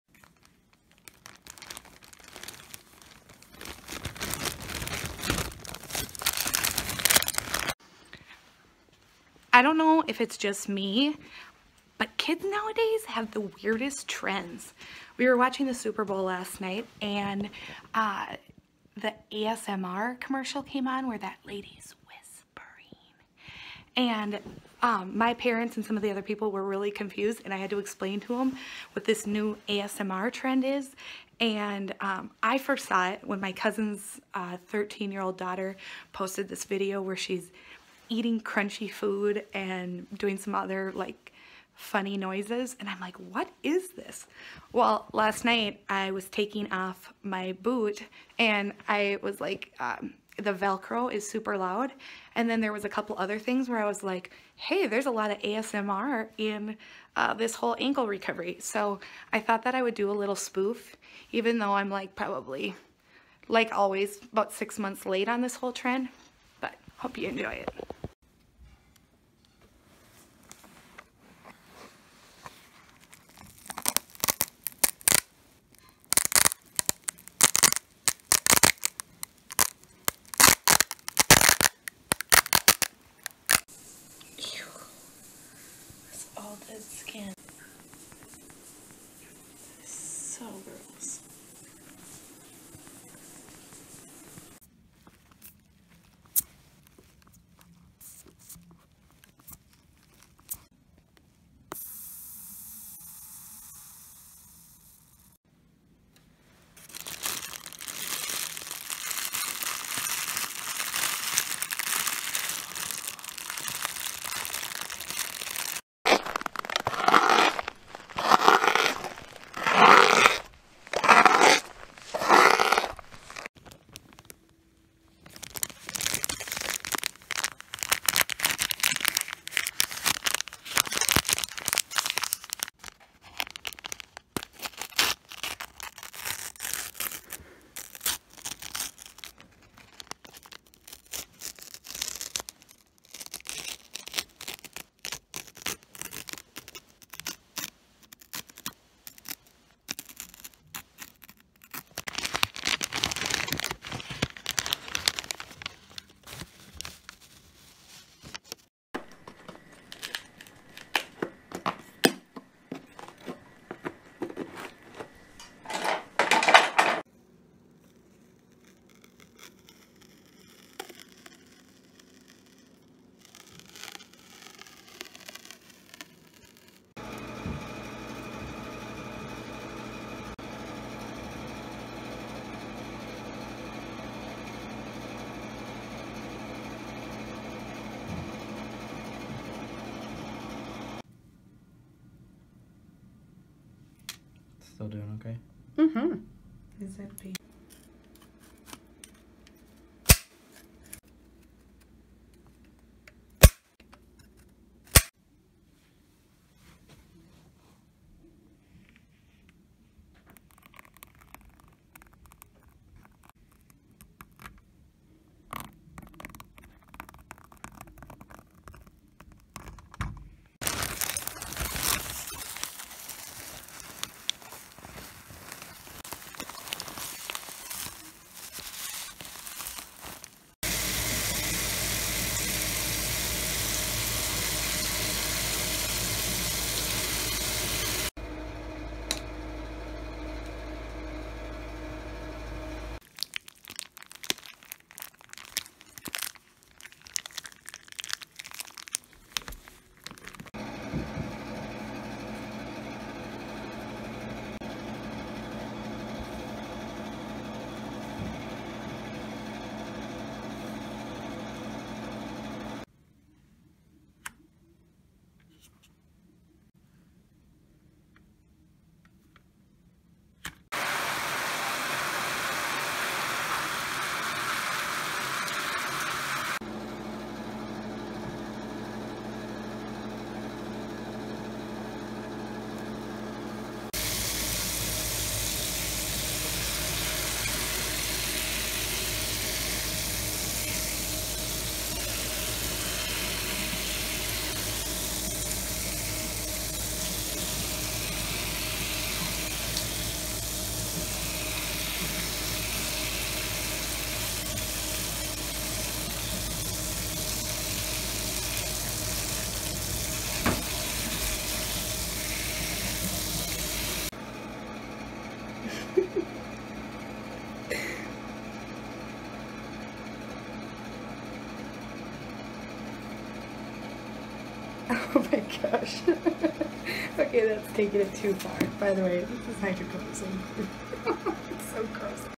ASMR - Broken Ankle Edition
Sights, sounds and feels dealing with ankle recovery.
Velcro of walking boot, scratching the pad of crutch pad, Vomit bag crunching, pumping air into air boot, hiss of deflating air, First Aid Gauze packet being ripped open, Iwalk on snow, Hydro cortisone cream, gel ice pack squeezed, Crutches adjustment, Cast cover getting rained on

Sarah House MN.